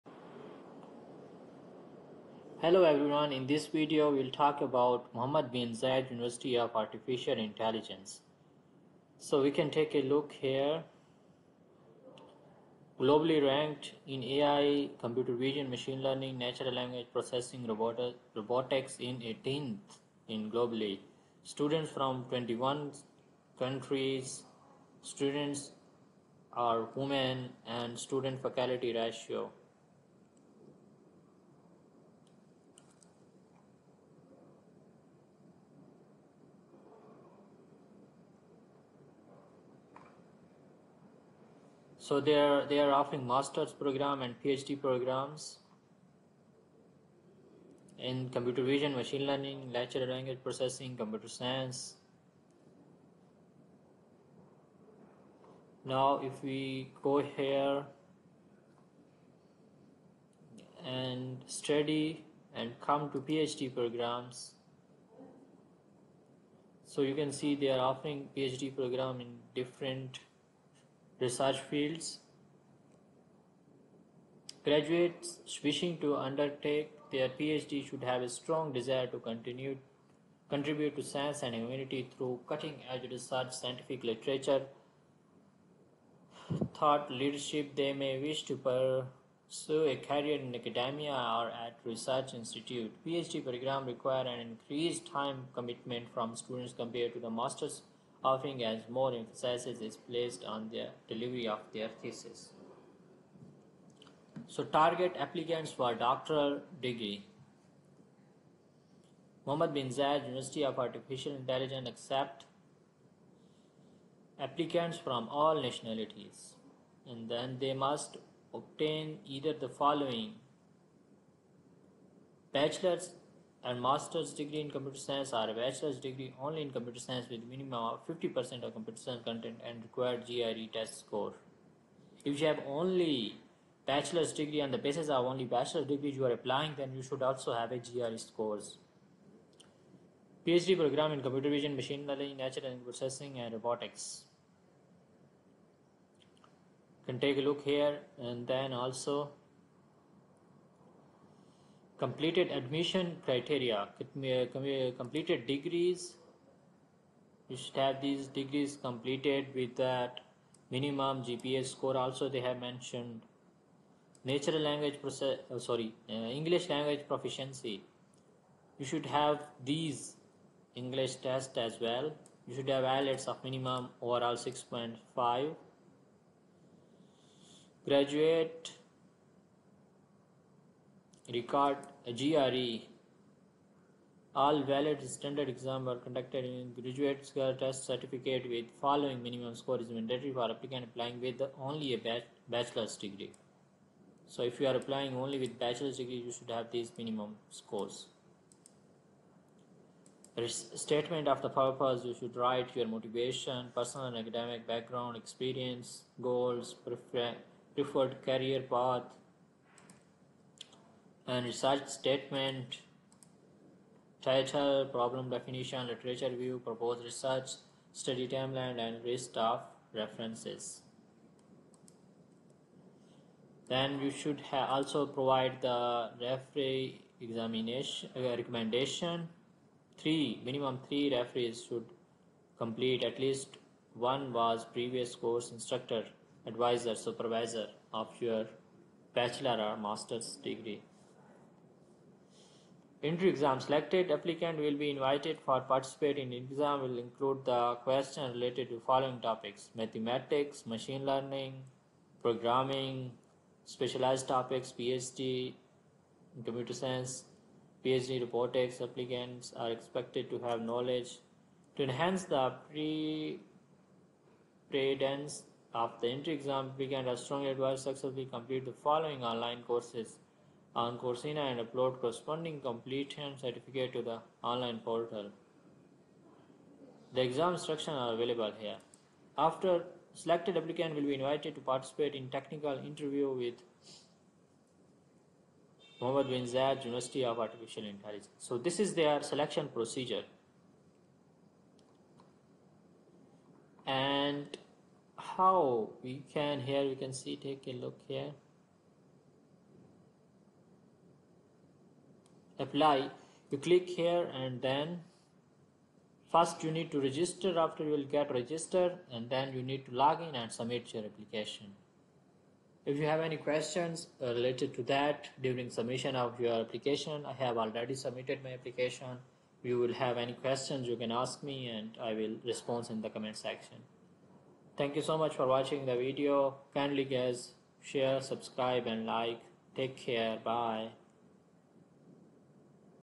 Mohamed bin Zayed University of Artificial Intelligence: Stepwise Procedure
📚 Scholarship Hunters Club 🌟

🎓 Explore Mohamed bin Zayed University of Artificial Intelligence: Stepwise Procedure

 In this video, we dive into the detailed step-by-step procedure for navigating the admission process at Mohamed bin Zayed University of Artificial Intelligence (MBZUAI). Whether you're a prospective student, researcher, or AI enthusiast, this guide will provide you with valuable insights into the application and enrollment process.

📋 Key Points Covered:

Introduction to MBZUAI: Learn about the prestigious Mohamed bin Zayed University of Artificial Intelligence and its commitment to advancing AI research and education.
Admission Requirements: Discover the essential criteria and documents needed to apply for programs at MBZUAI.
Application Process: A walkthrough of the application process, including important deadlines and submission guidelines.
Programs Offered: Explore the various AI-focused programs available at MBZUAI and find the perfect fit for your academic and professional goals.
Scholarship Opportunities: Information on available scholarships and financial aid to support your journey at MBZUAI.
Campus Life: Get a glimpse into the vibrant campus life, cutting-edge facilities, and the supportive academic community at MBZUAI.
Q&A Session: Answers to frequently asked questions about MBZUAI and the application process.

🤝 Join us on this informative journey, and empower yourself with the knowledge needed to step into the world of artificial intelligence at Mohamed bin Zayed University!